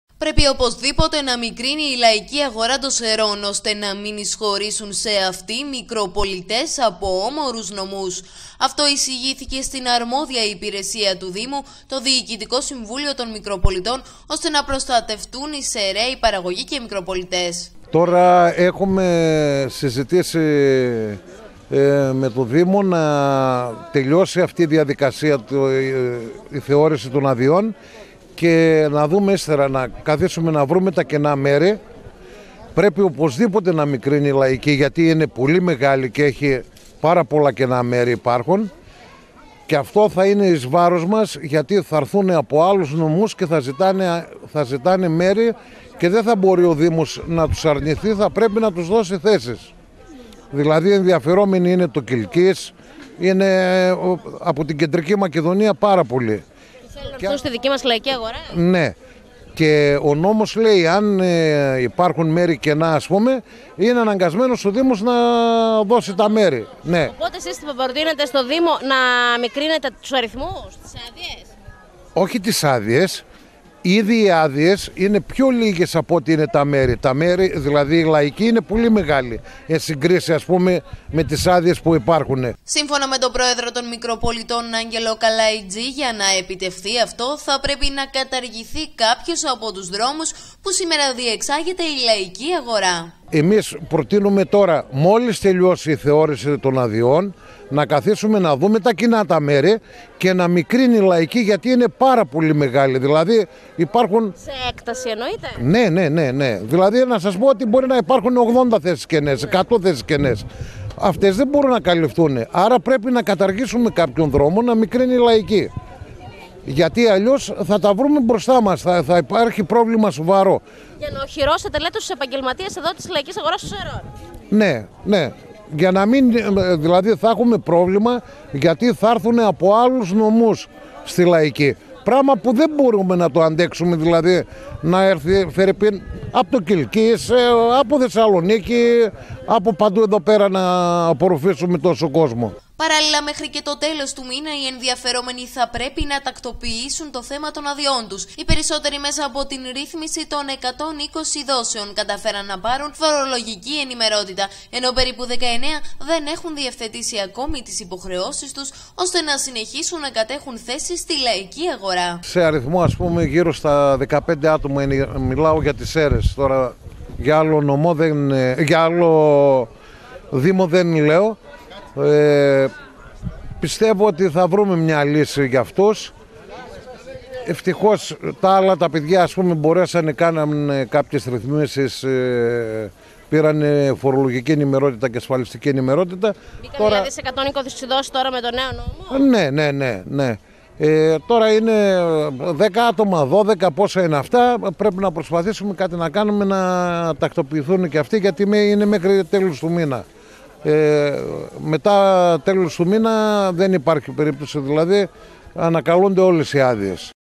Μεγάλη η λαϊκή σε σχέση με τις υπάρχουσες άδειες
Α.Καλαϊτζής: "Να μικρύνει η λαϊκή αγορά των Σερρών"